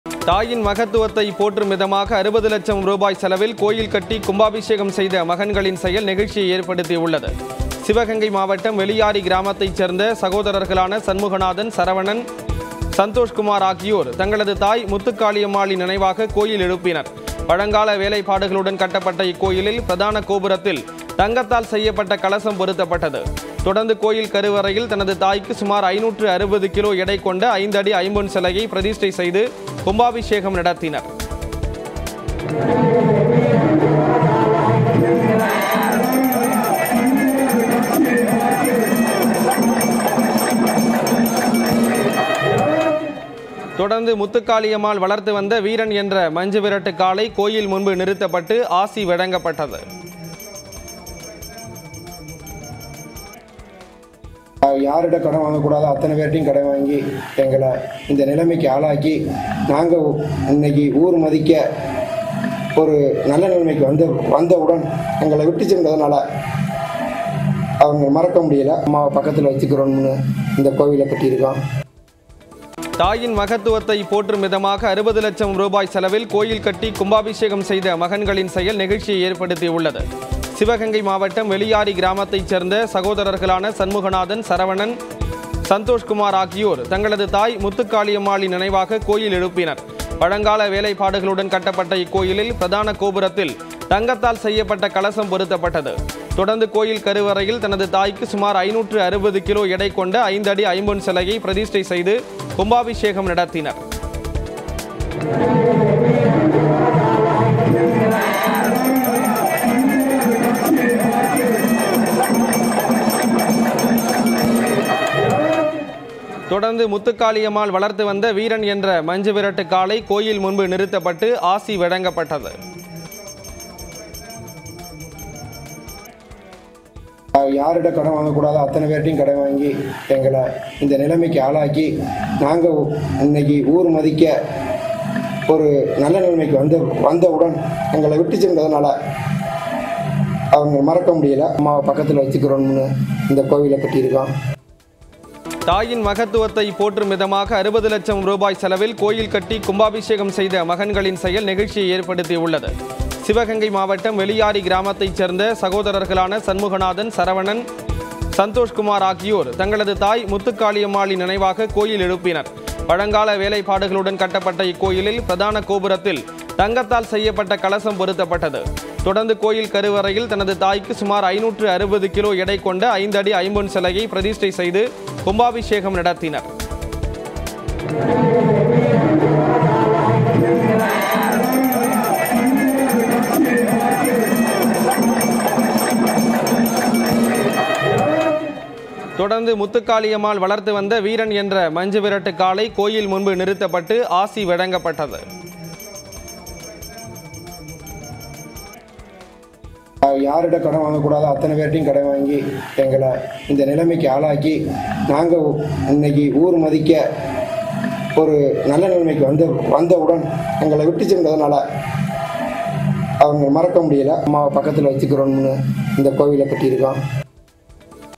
அம்மாவுக்கு 60 லட்சத்தில் கோவில்..மகன்கள் செய்த நெகிழ்ச்சி செயல்-சிலிர்த்துப்போன சிவகங்கை மக்கள்
தாயின் மகத்துவத்தை போற்றும் விதமாக அறுபது லட்சம் ரூபாய் செலவில் கோயில் கட்டி கும்பாபிஷேகம் செய்த மகன்களின் செயல் நெகிழ்ச்சியை ஏற்படுத்தியுள்ளது.